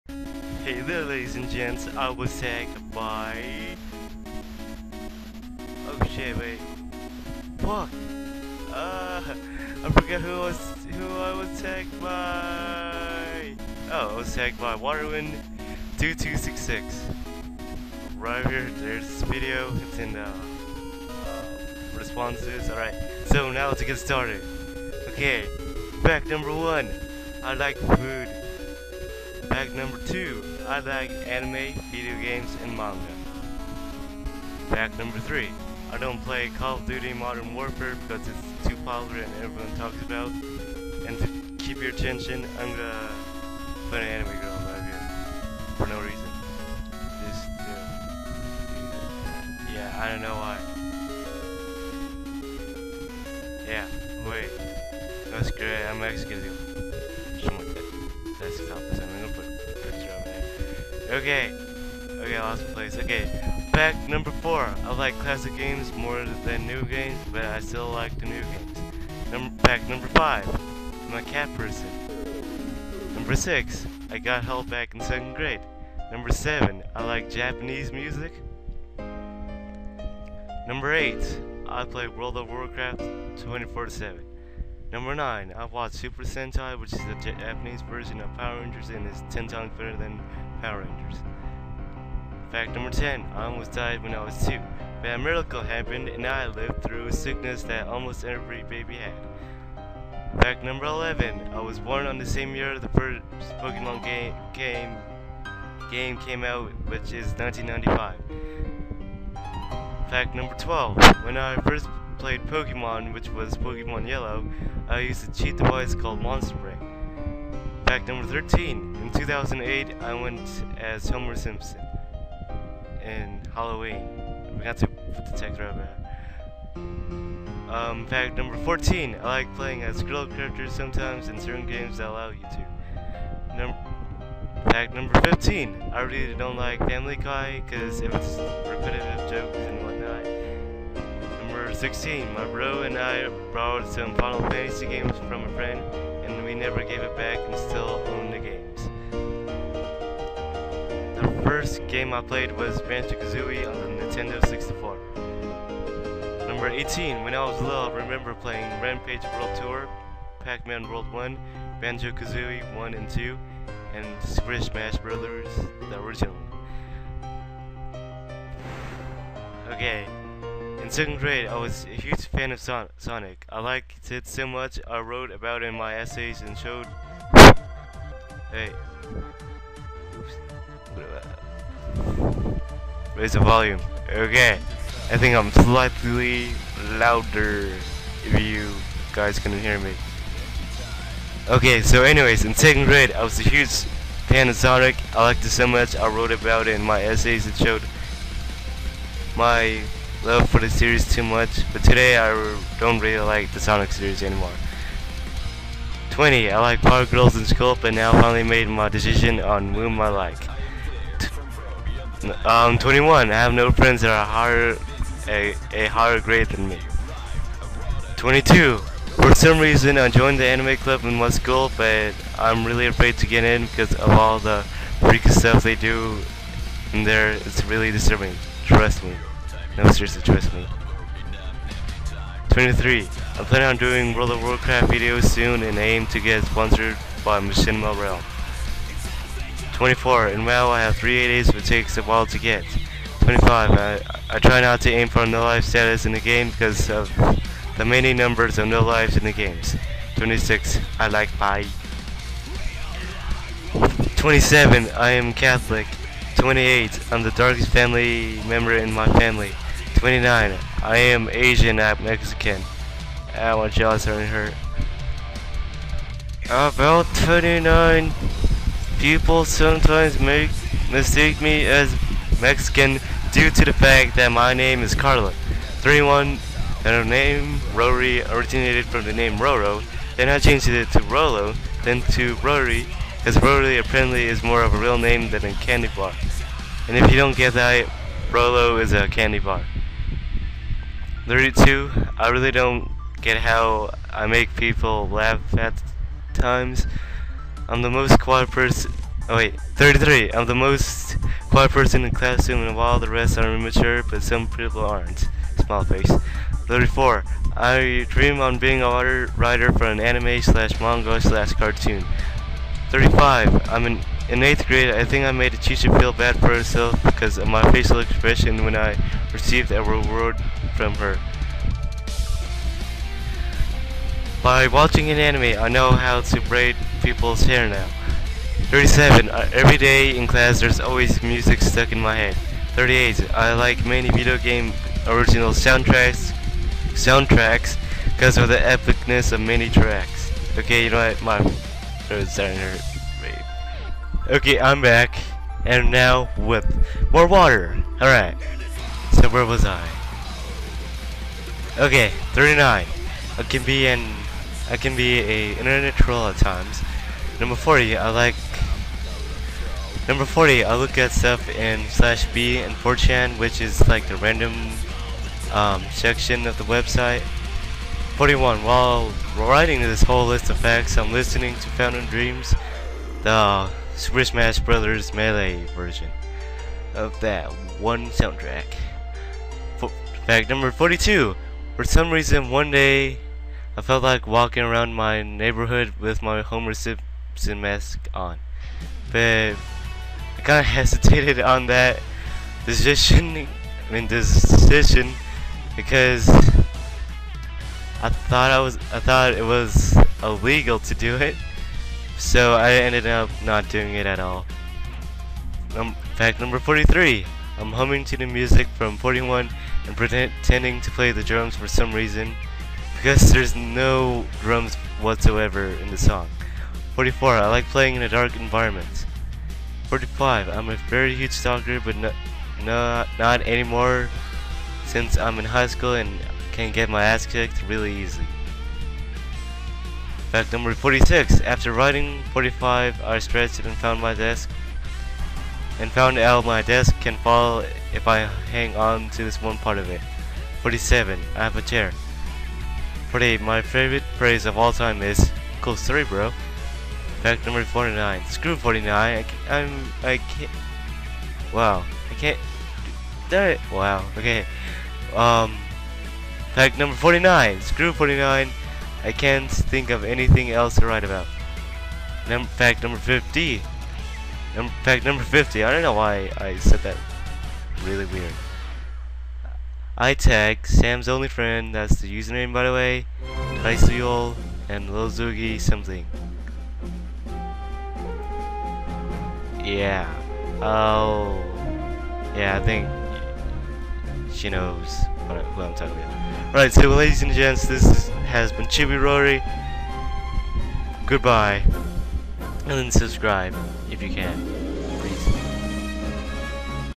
Tagged!? 50 Facts about me
Ya apparently i got tagged by Waterwind2266, but i 4got to save the file where i wrote the facts on, so ya just try your best to listen to me srry xD